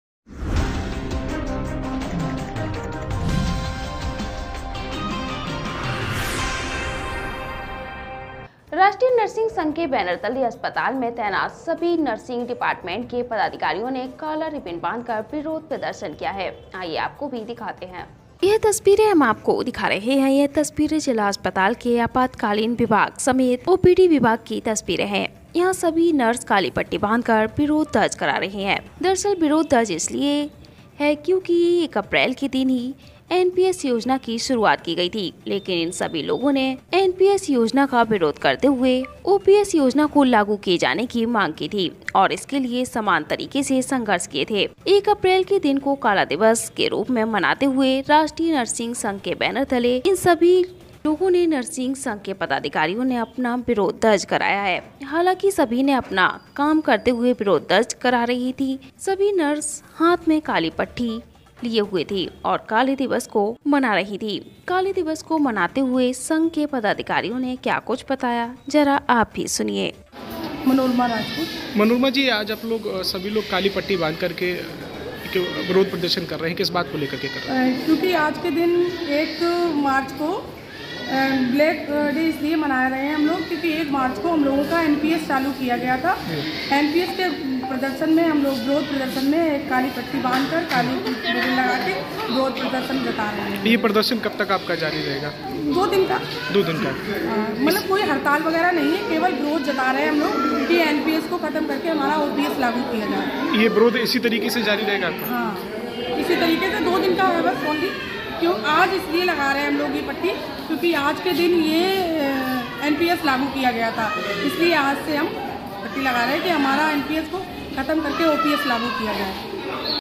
नर्सों ने 1 अप्रैल को मनाया काला दिवस के रूप में
काली पट्टी बांधकर जताया विरोध